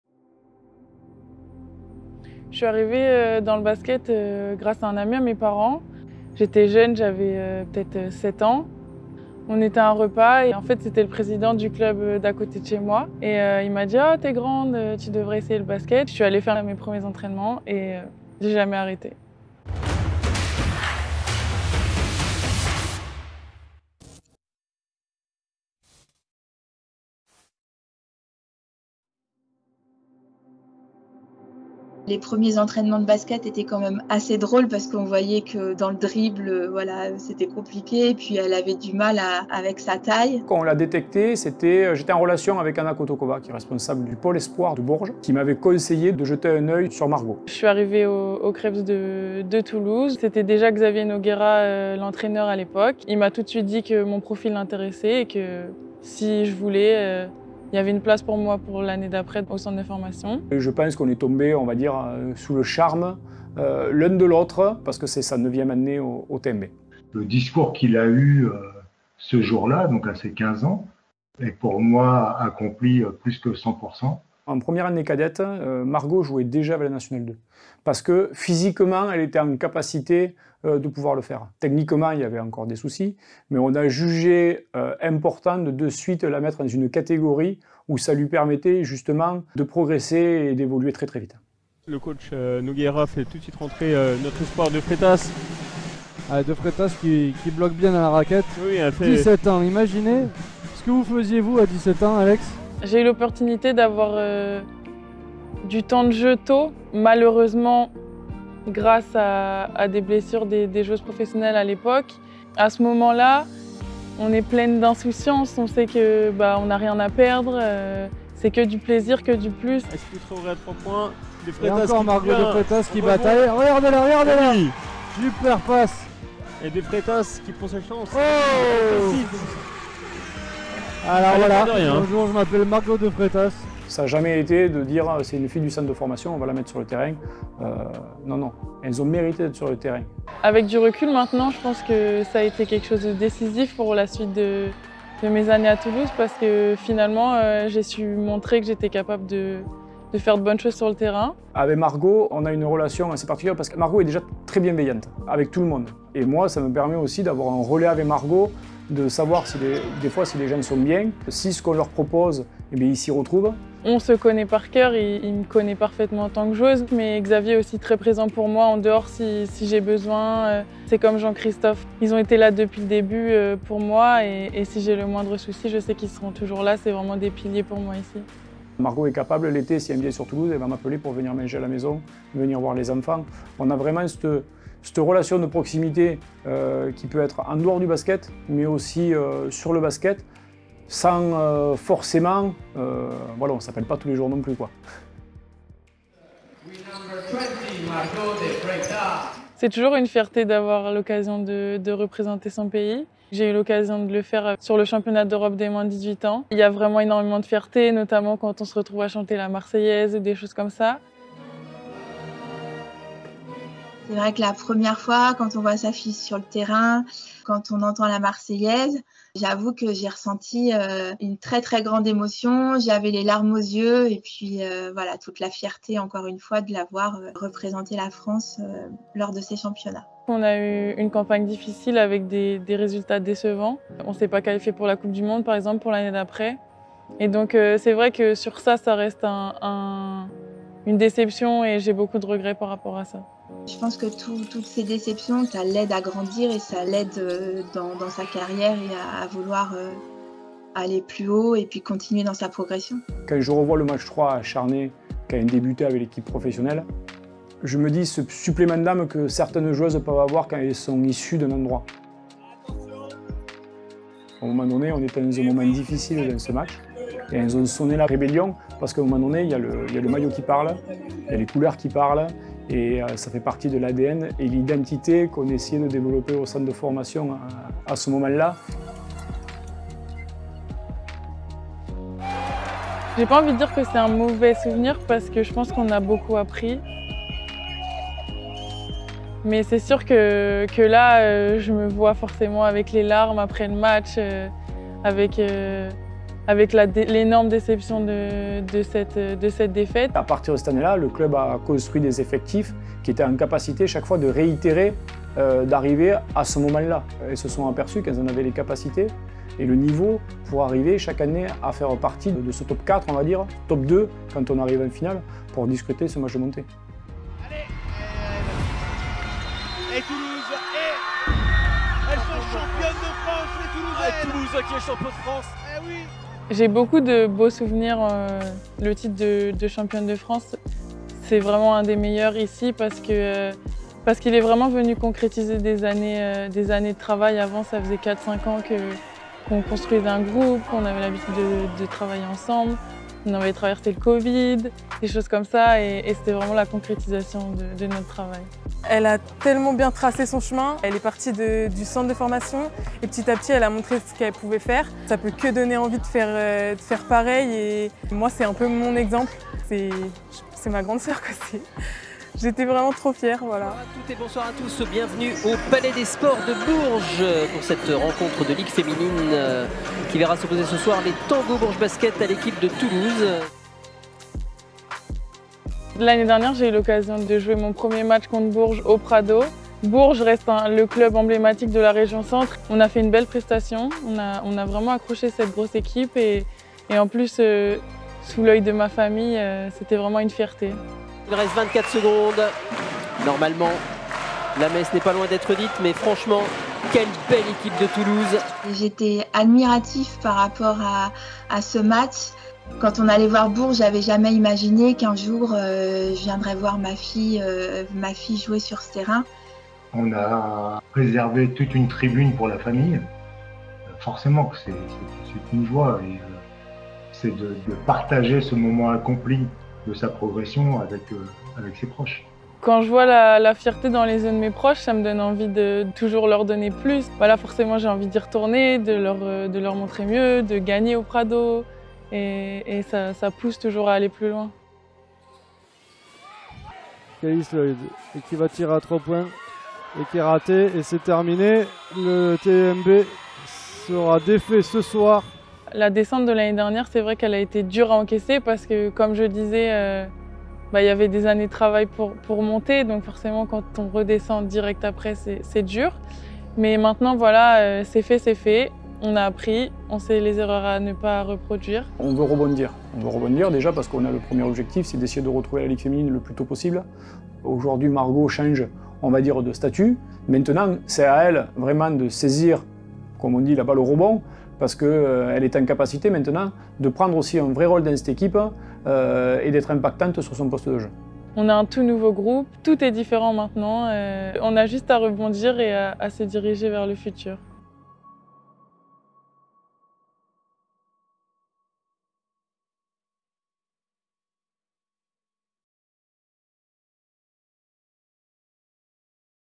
Coast-to-Coast Ep.01 - Margot De Freitas
Coast-to-Coast, une production TMB x La Fabrique Visuelle x Fit Group 🎬

Découvrez à travers cette web-série nos Pionnières durant cette saison 23/24.

Épisode 01 : Margot De Freitas